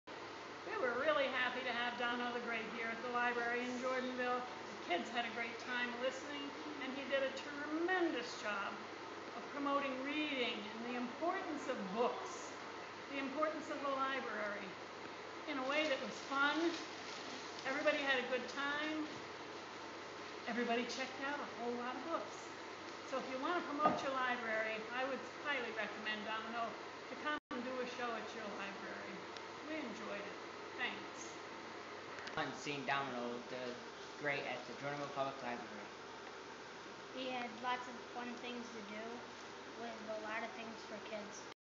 Herkimer County NY Magician_Jordanville New York Summer Library Reading Program Review 7.mov
A very happy librarian summer reading review regarding Herkimer County Magician & Magical Storyteller: Domino The Great. The parents and kids in Jordanville New York loved Domino's Splish Splash Reading Show!!!